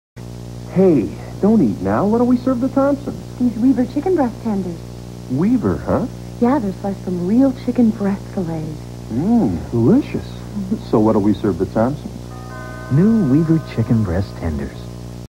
1987 Weaver's commercial
Quick spot for the premium chicken tenders from April 1987.